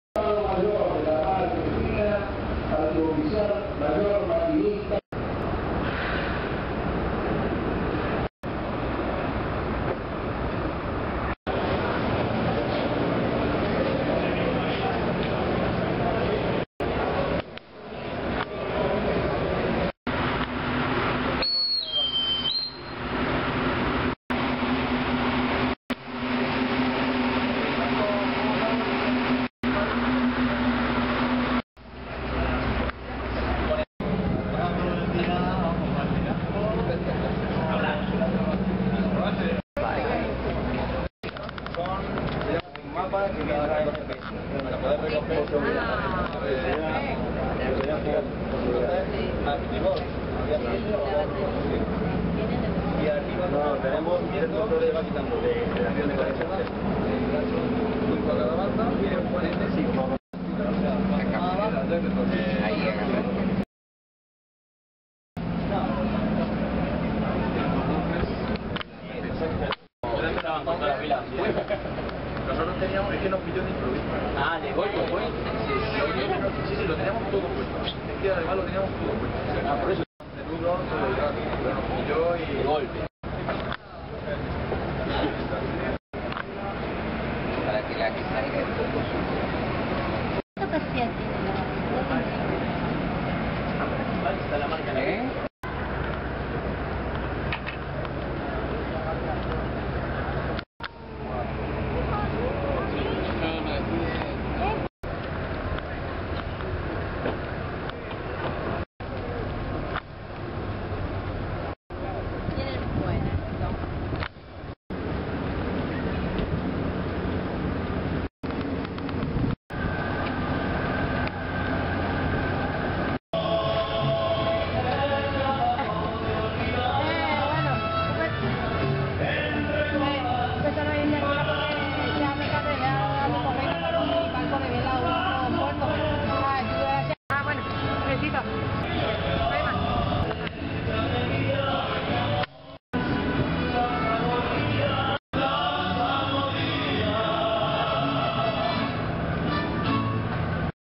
Velas Sudamerica 2010, Part 1, 5 Mar 2010
5 March 2010 and there are 10 windjammers in Buenos Aires as follows:  IN  DARSENA  NORTE -   Barques CUAUHTEMOC of Mexico and SAGRES of Portugal  on the NE Wall or Baliza Nordeste.  Next the two Brigantines, ESMERALDA of Chile and JUAN SEBASTION de ElCANO of Spain along side the Naval War College.  On the west bulkhead, the Full Rigged Ships ARA LIBERTAD of Argentina and CISNE BRANCO of Brasil.  Finally, on the south bulkhead east of the old lock to Dique 4 we find the Barque SIMON BOLIVAR of Venezuela.      IN DIQUE 4 of PUERTO MADERO - Along the west bulkhead the Barque GLORIA of Colombia, Goleta CAPITAN MIRANDA of Uruguay and Barque Guayas of Ecuador.